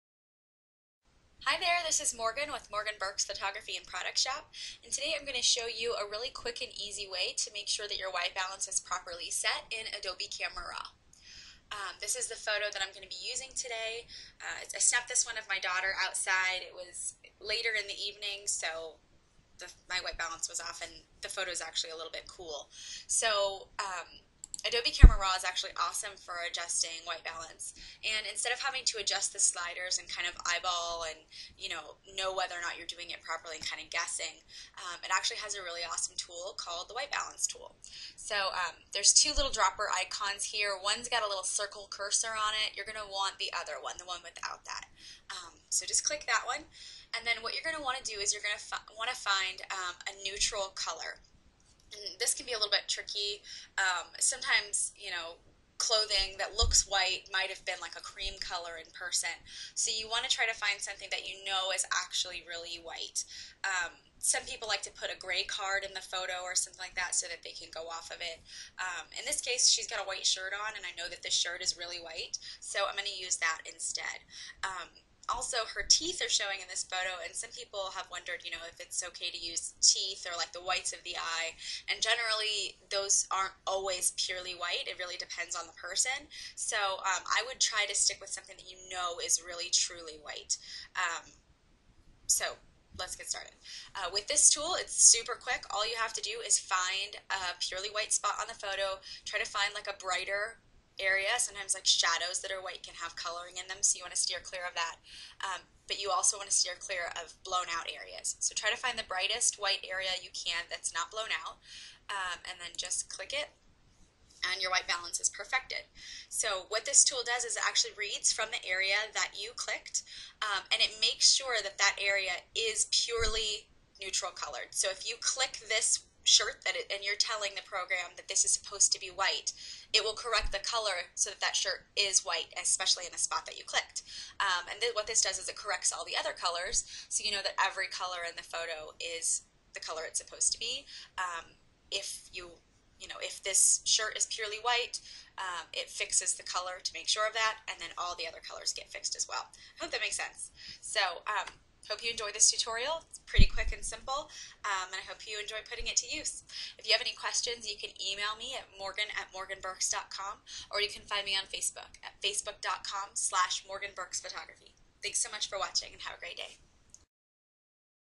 How to Use the White Balance Tool in Adobe Camera RAW
This video will show you how to achieve a proper white balance with just one click of the White Balance Tool in ACR.